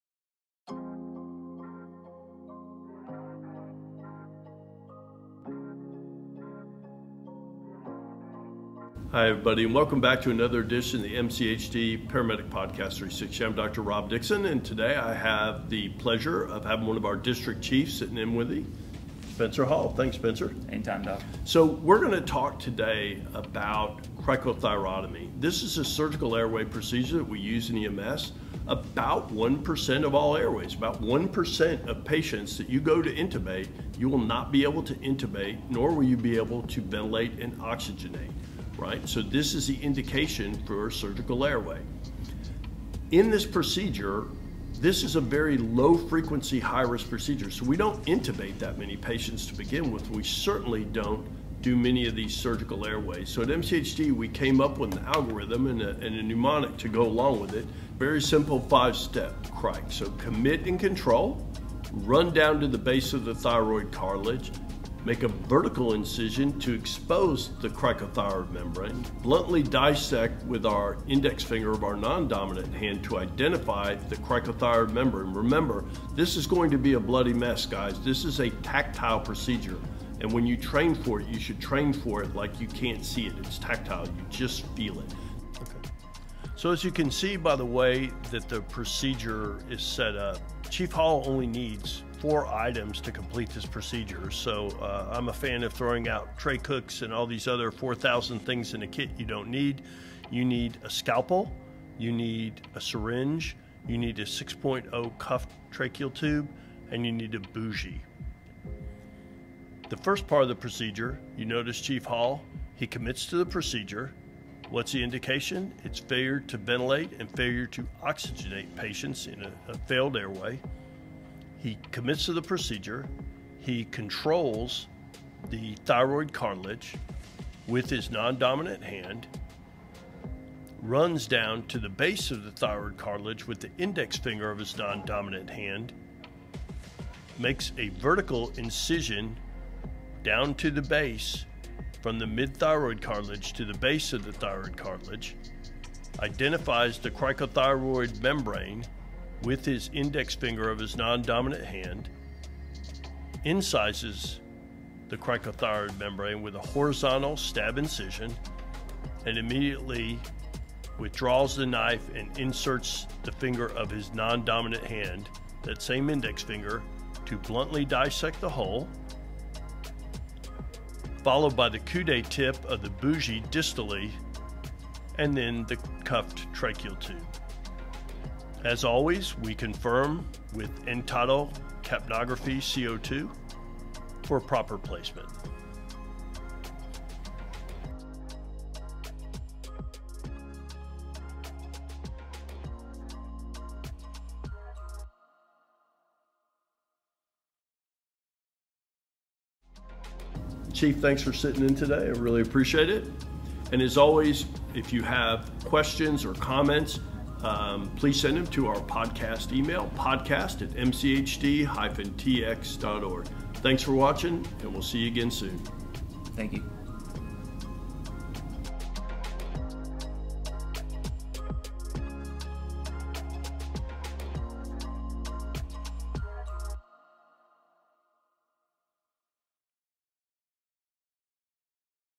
MCHD Paramedic Podcast 360: Creating a Surgical Airway
Join MCHD Medical Director Dr. Robert Dickson & District Chief Spencer Hall as they teach our protocol for CRIIC, our term for creating a surgical airway in a patient. Please note, this was filmed pre- COVID-19.